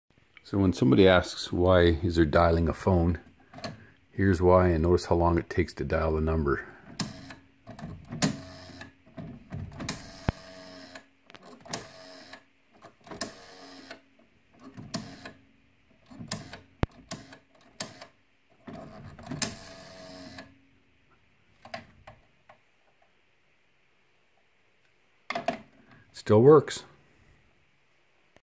Old school dial phone - Things you see while showing homes
I was showing a home today and funny I forgot how long it took to dial a number.